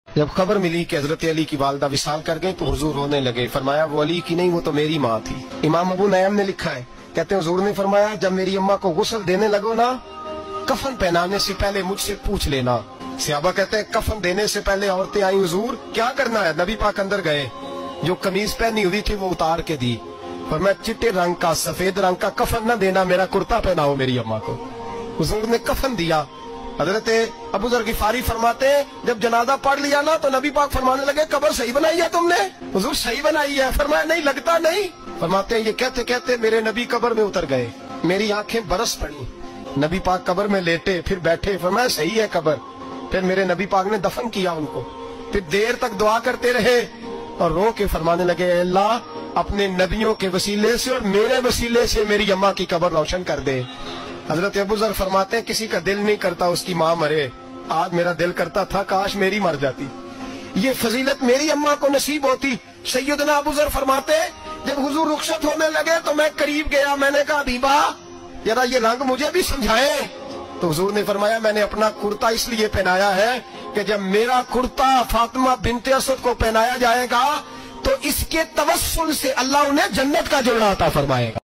Hazrat Ali RZ ki Walda ka Jab intiqaal Hoa || peer Ajmal Raza Qadri Bayan || Islamic video’s 2024
Peer Ajmal Raza qadri bayan 2024 || Islamic video’s 2024#viral